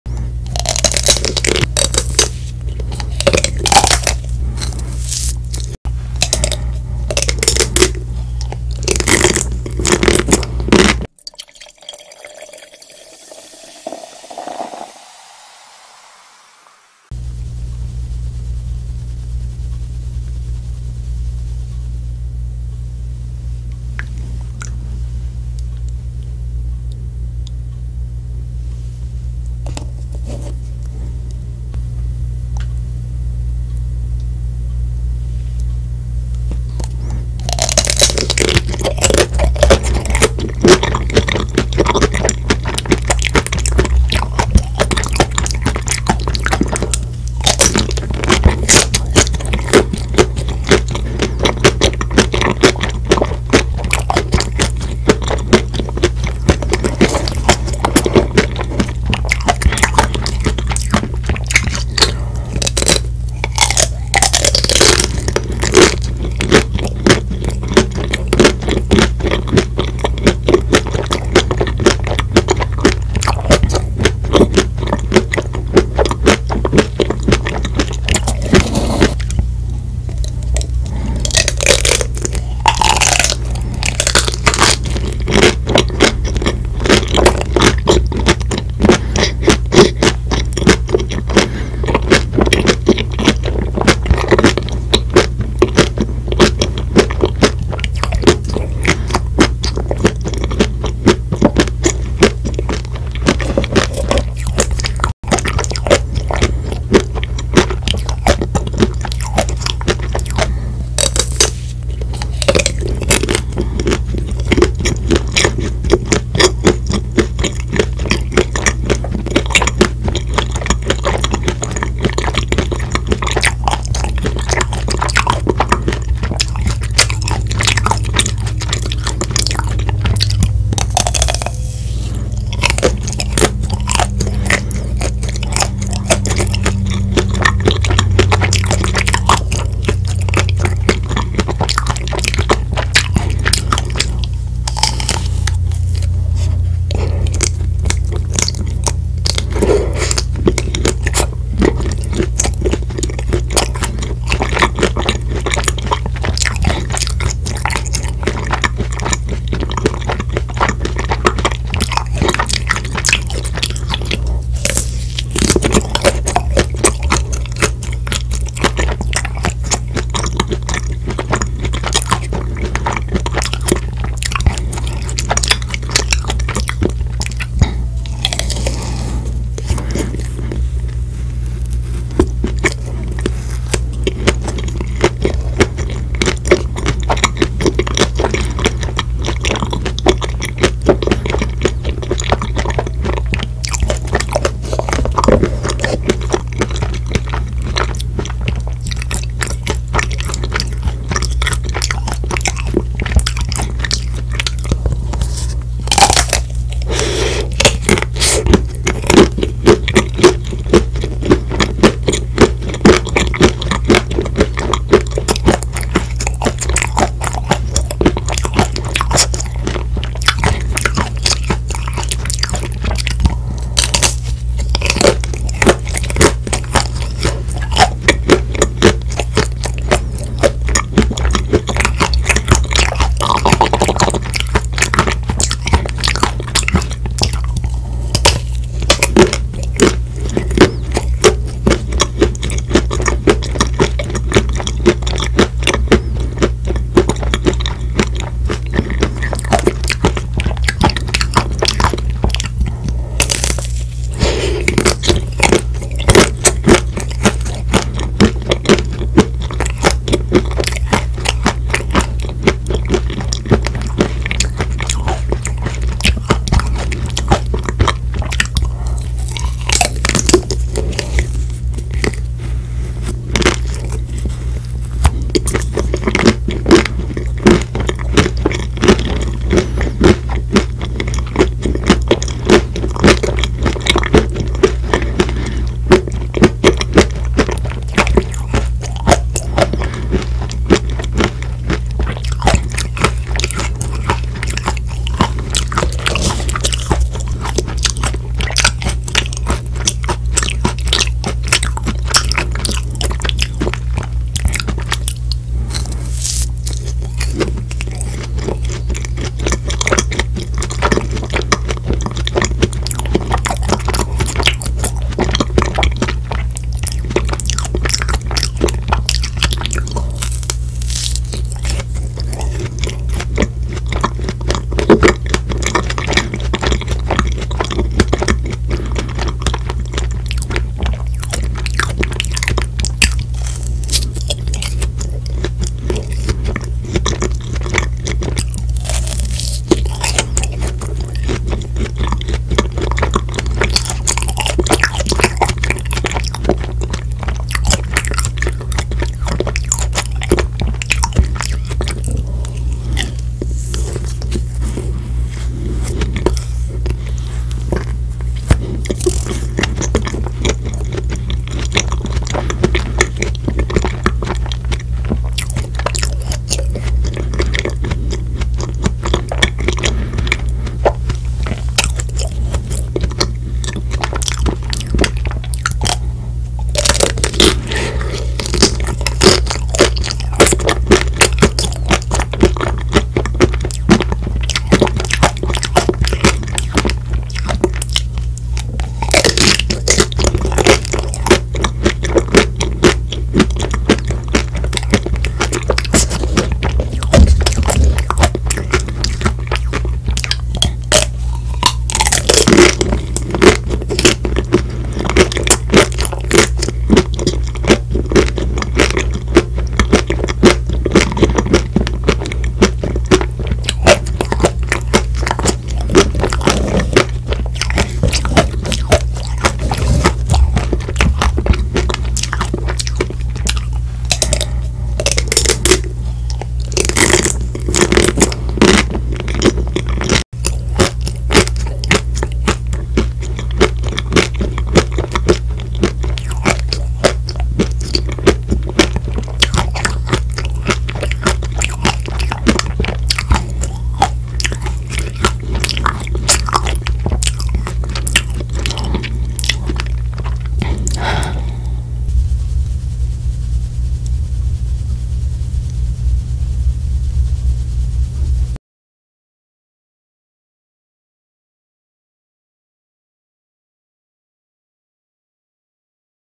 ASMR CHOCOLATE & CREAM CHEESE ICE CREAM*CLEDOR Eating 끌레도르 초콜릿 & 크림치즈 아이스크림 먹방 리얼사운드
💜끌레도르 쿠키앤크림, 끌레도르 크림치즈 레몬, 끌레도르 크림치즈 블루베리, 티코 다크초코

💜Cledor Cookie&Cream, Cledor Cream Cheese Lemon, Cledor Cream Cheese Blueberry, Tico Ice Cream Dark Chocolate

⭐영상은 매주 월, 수, 금, 일요일 저녁 7시에 업로드합니다
This channel will be upload in every Monday, Wednesday, Friday and Sunday at 7:00pm